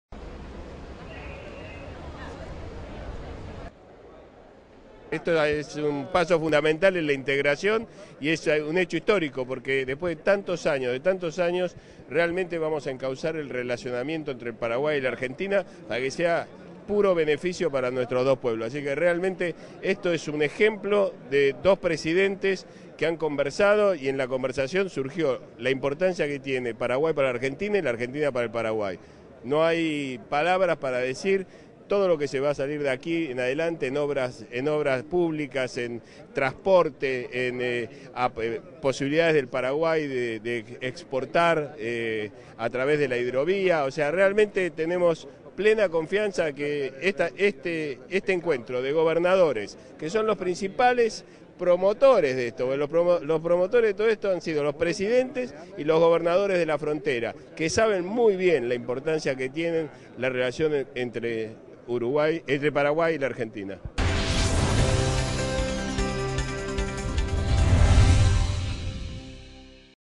Timerman: "Este encuentro es un hecho histórico para las relaciones entre Paraguay y la Argentina"
El canciller Héctor Timerman destacó la trascendencia del encuentro realizado hoy entre los gobernadores de la región fronteriza entre Argentina y Paraguay y calificó a la reunión como un "paso histórico" para el relacionamiento entre ambas naciones.
"De este encuentro van a surgir posibilidades de desarrollo y crecimiento enormes para los dos países como nunca antes ocurrió" afirmó Timerman y sostuvo que los principales protagonistas del encuentro fueron "los presidentes de ambos países y los gobernadores de la región fronteriza porque ellos saben mejor que nadie la importancia del relacionamiento bilateral"